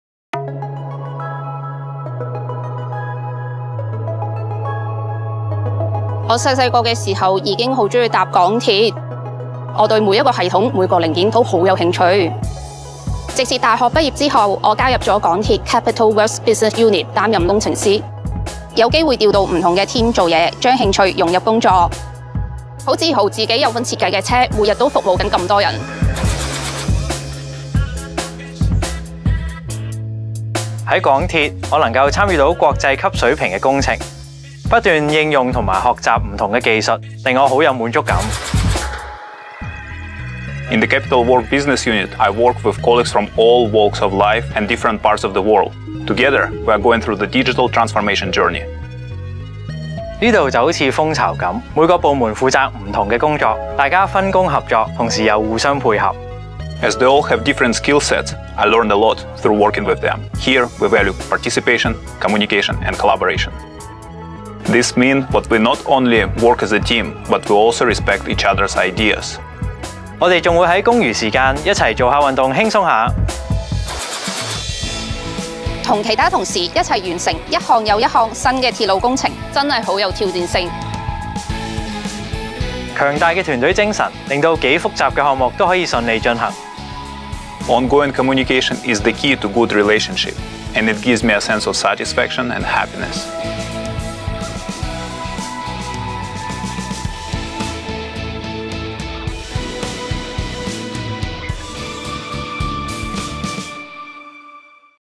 Building Railways ‧ Building Careers
Various network extension projects including the Tung Chung Line Extension, Northern Link, Tuen Mun South Extension, Siu Ho Wan Station and Hung Shui Kiu Station will be carried out progressively. Interested in being part of the team to participate in the new projects from design stage as well as a myriad of improvement projects? Act now!